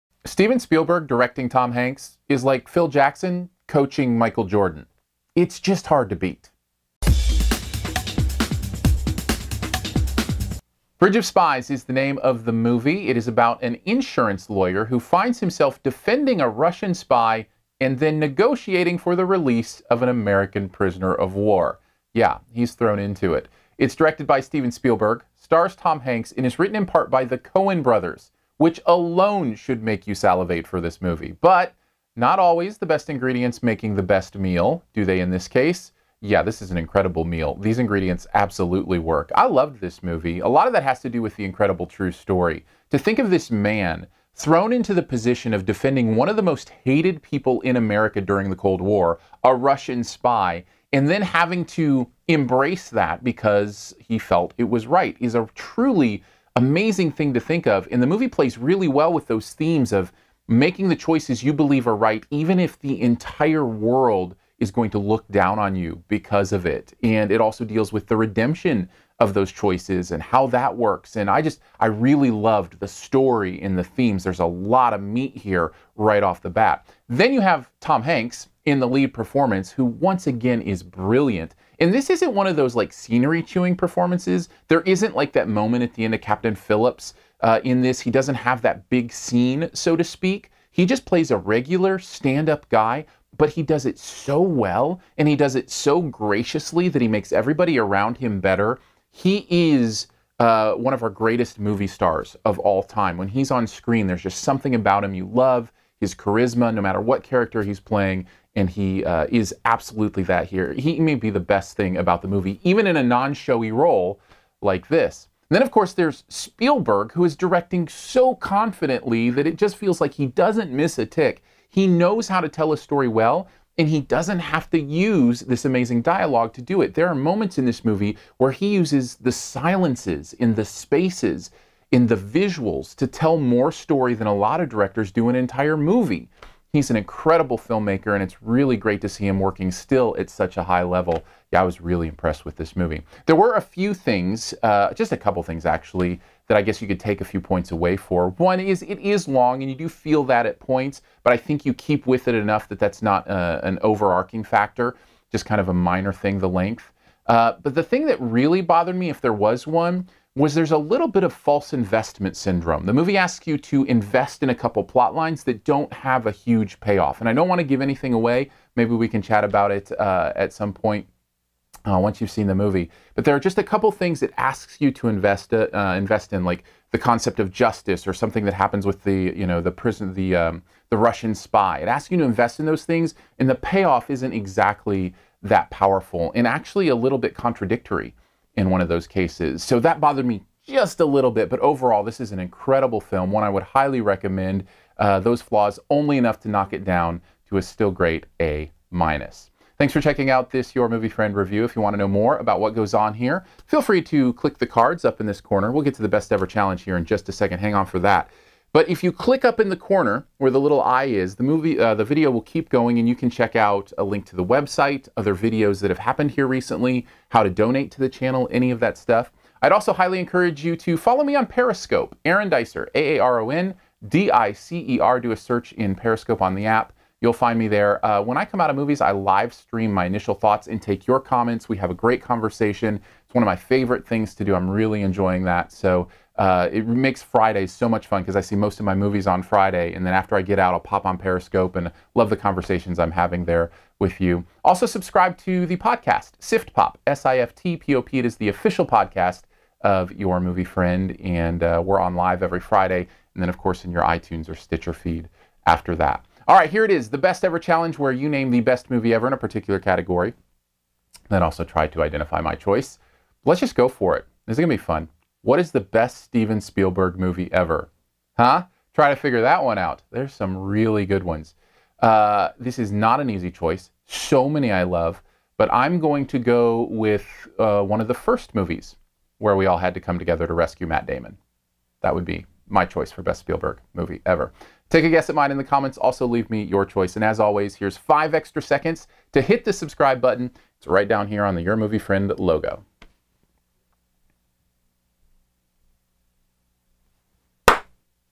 Bridge of Spies - Movie Review (Your Movie Friend)
The best thing, worst thing, and main thing about "Bridge of Spies" spoiler free and in about the time it takes to watch the trailer.

*depending on number of films released

Follow @aarondicer on Periscope for immediate interactive reviews of every single movie released!!

or search "SiftPop" in your favorite podcast player.